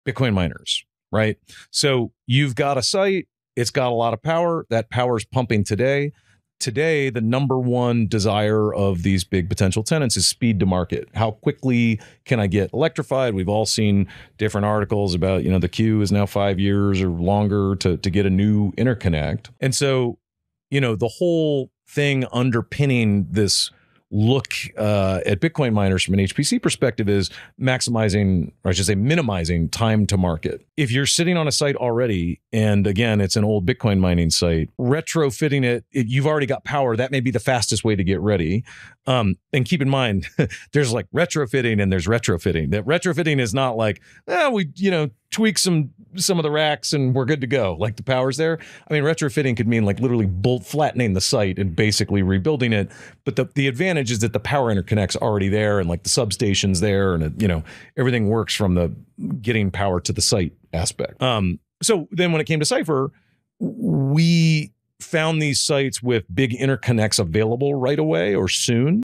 Bitcoin Mining Sites: Fast Power & Speed-to-Market
Bitcoin mining sites offer a surprising advantage. Repurposing an existing site minimizes time-to-market. The power interconnect is already there, so electrification is faster. Even with major retrofitting, it's still the quickest path.